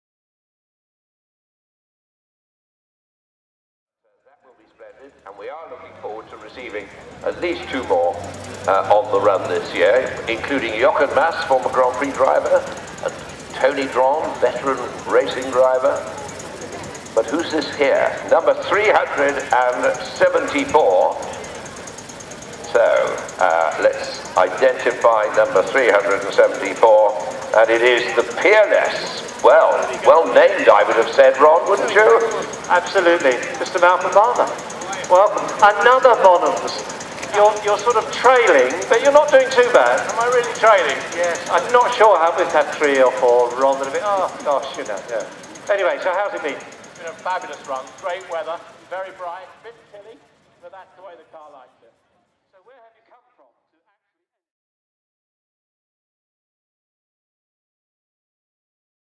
london to brighton veteran car run 2010
lbvcr... A maximum 550 entries have been received for the 2010 Run. A lovely dry crisp sunny day.